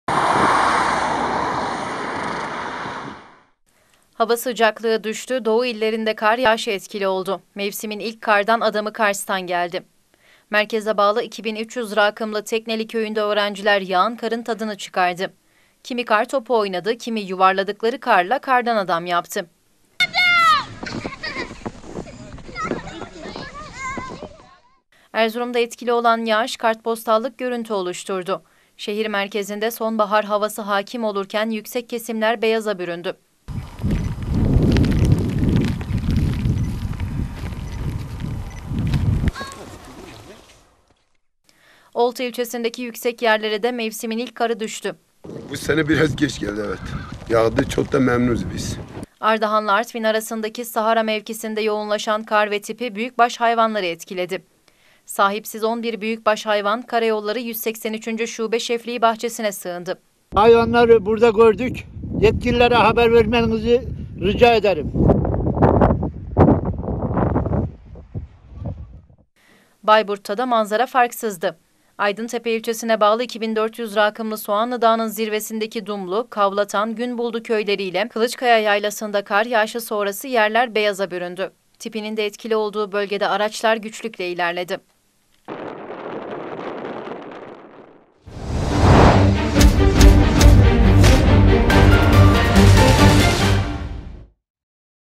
Kardelen TV | Doğu Kara Teslim Oldu
Meteoroloji uyardı… Hava sıcaklığının düşmesiyle birlikte doğu kar yağışı etkisi altına aldı. Erzurum, Kars, Ardahan ve Bayburt’tan kar manzaraları geliyor ekranlarınıza…